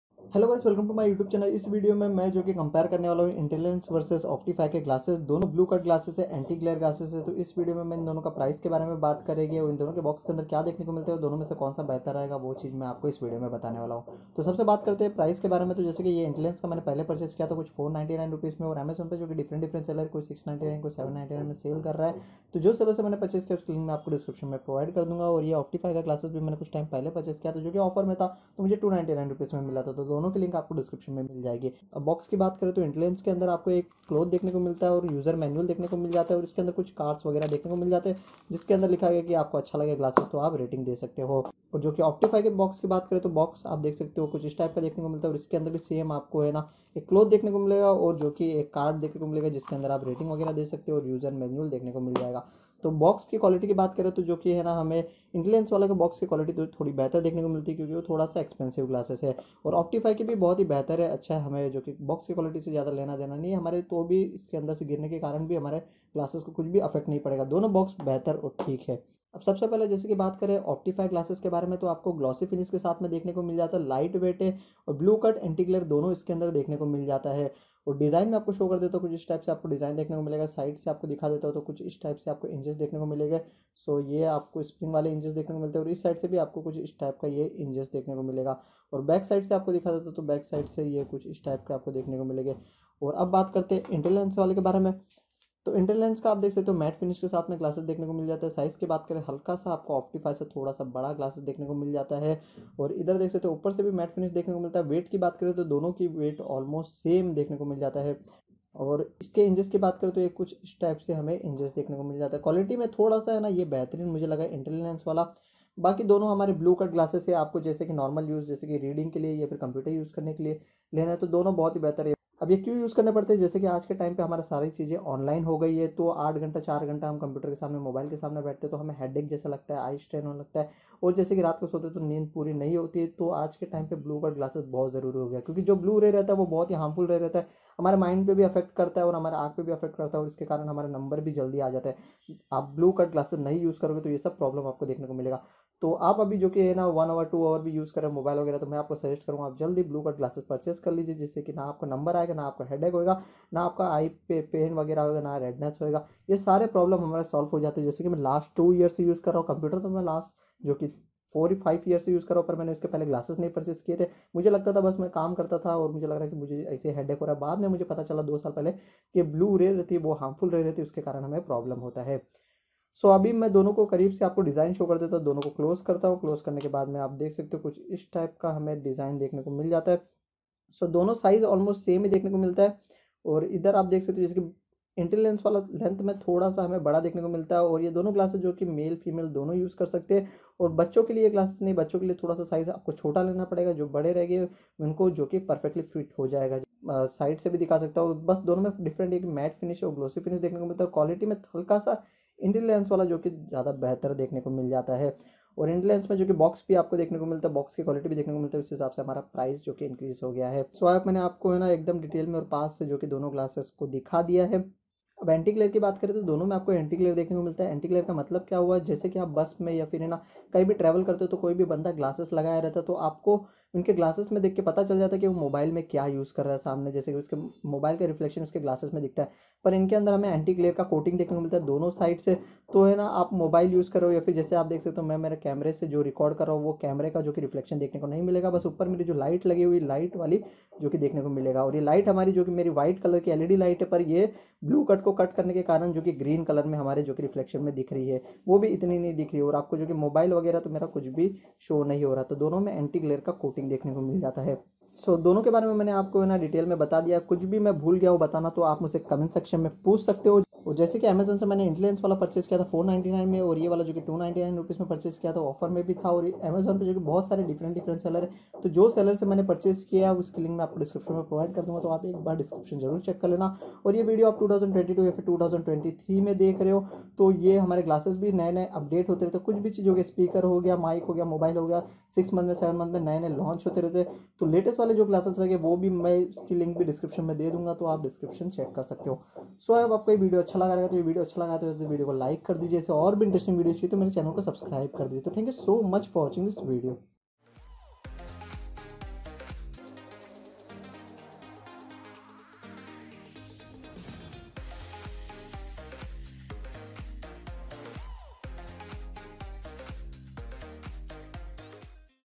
Intellilens® VS Optify® Unisex Blue Ray Cut and Anti Glare Zero Power Unboxing and review.(2021)
🖥️ Led Monitor
💻 Laptop which I use for Video editing
🎤Boya Microphone
🎤Another Blue Snowball iCE USB mic